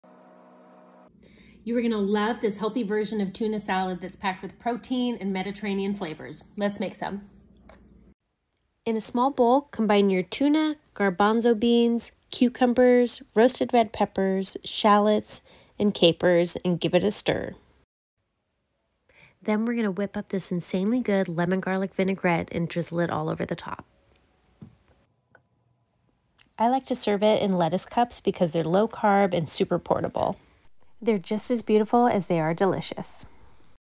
Mediterranean Tuna Salad
You'll love this healthier version of tuna salad, lightened up with a mayonnaise-free lemon garlic vinaigrette. The tuna is bursting with Mediterranean flavors like roasted red peppers, capers and cucumber. You'll want to eat this for lunch every day!